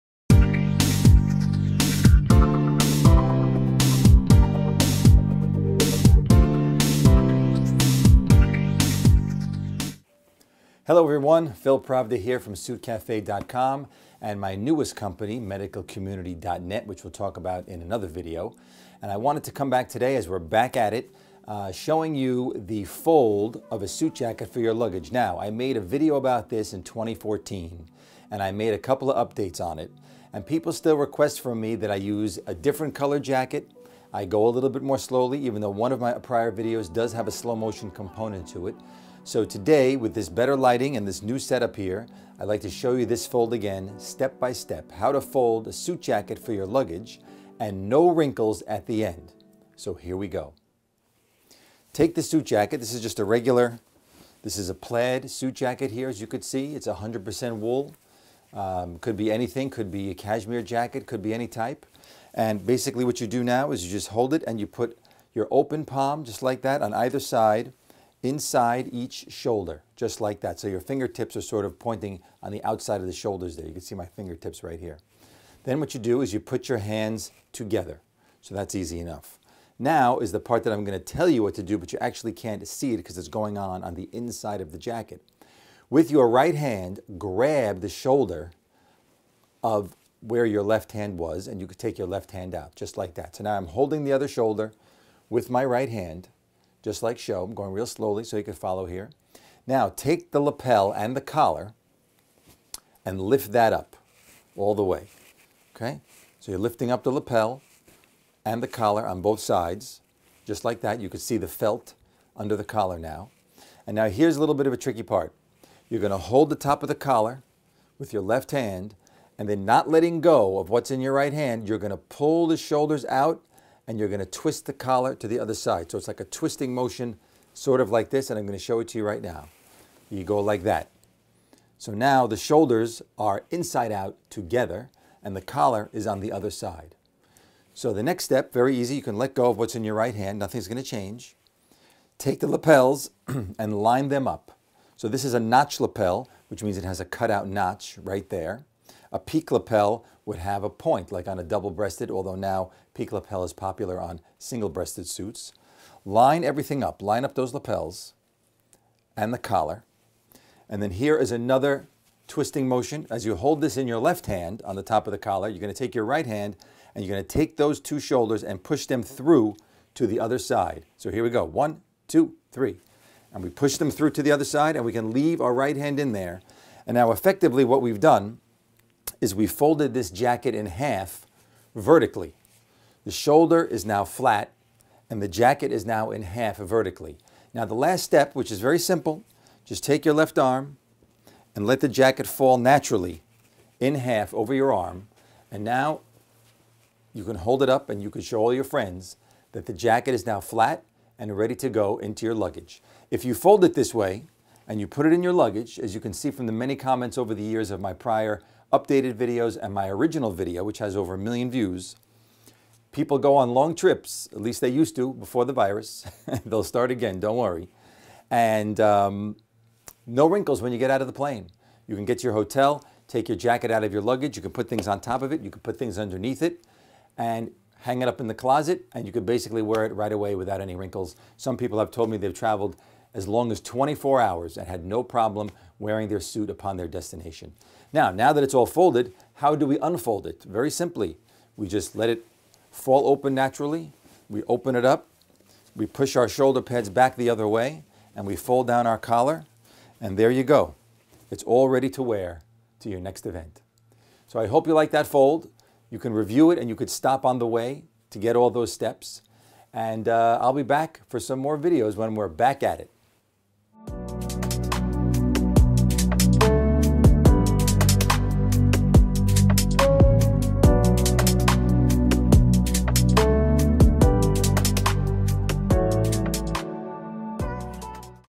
BEST How To Fold A Suit Jacket For Your Luggage *NEW UPDATE* NO WRINKLES 2021
Back in 2014 I made the first video for SUITCAFE about a 4th generation fold for a suit jackets handed down throughout the family. Millions of people watched that video and I have done updates. Here is a 2021 update that hopefully you all enjoy and can easily follow along.
Duet me on Tik Tok with this fold: @suit_cafe
Think you can do it as fast?

Protective face masks made in a menswear clothing factory. Better fit and fabric for everyday use.